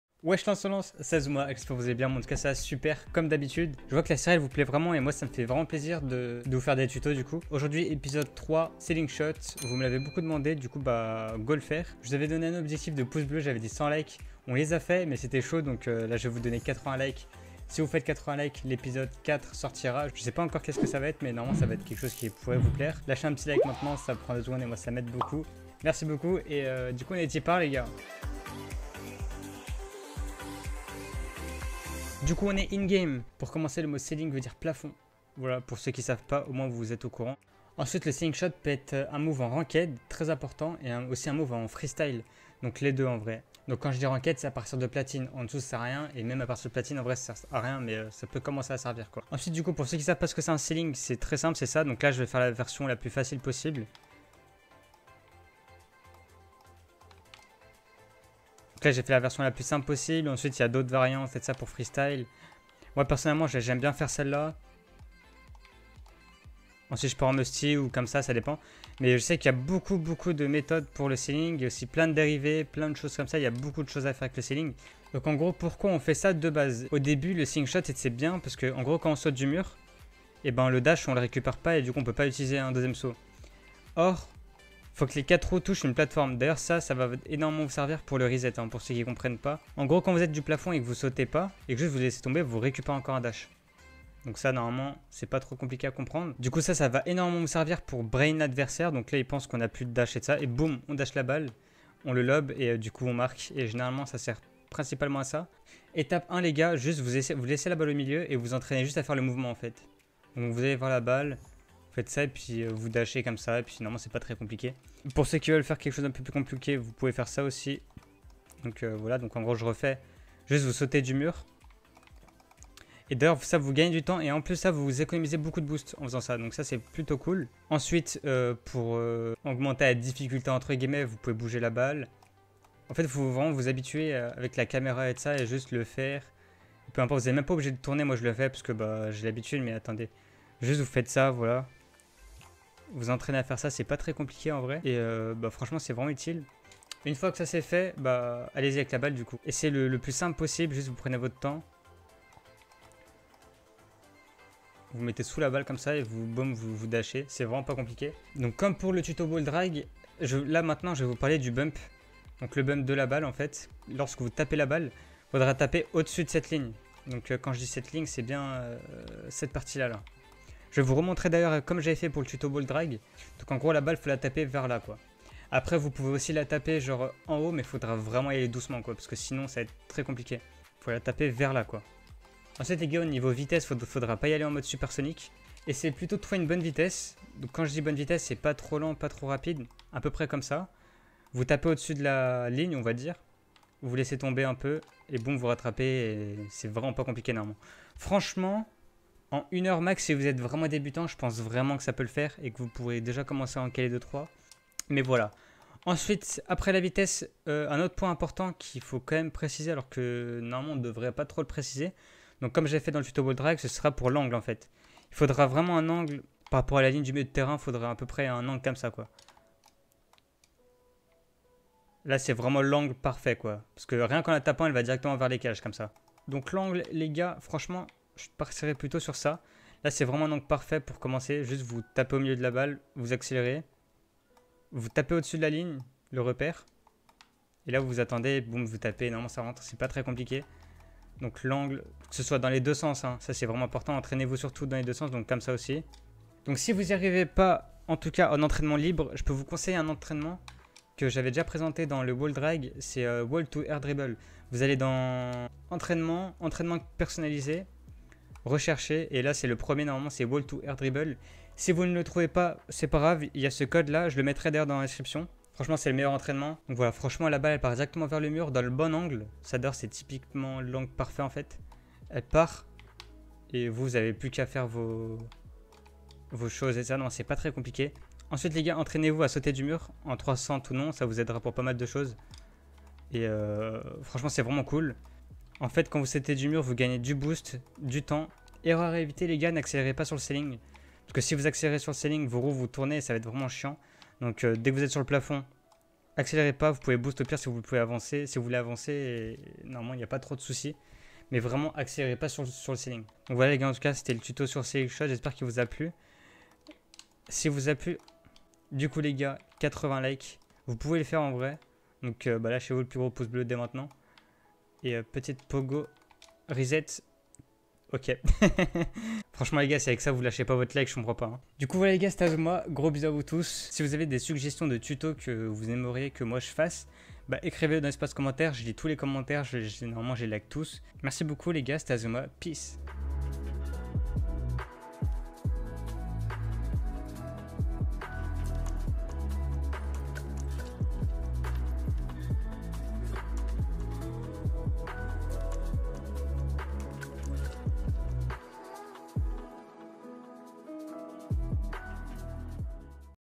Faire un CEILING SHOT ! (plafond) - TUTO FREESTYLE #3 / Azuma
⚠️ Il n'y a que TOI qui peux m'aider booster ma chaîne.

► Code Créateur : 𝗔𝗭𝗨𝗠𝗔
Merci pour les 6168 abonné(e)s ! 😄

► FRANÇAIS ◄
Salut !
Aujourd'hui, je vous ai fait
un tuto freestyle comme vous
me le demandiez depuis des mois.
Aujourd'hui: Episode 3: le ceiling shot
(partir du plafond)
En espèrant que ça vous a plu !
PEACE ! 🙂

► ENGLISH ◄
Hello !
Today i made you
a freestyle tutorial like you
been asking me for months.
Today: Episode 3: the ceiling shot
Hope you enjoyed it!
PEACE! 🙂

Camera Settings:
Field of view - 110
Distance - 270
Height - 100
Angle - -4
Stiffness - 0.40
Swivel Speed - 6.00
Transition Speed - 1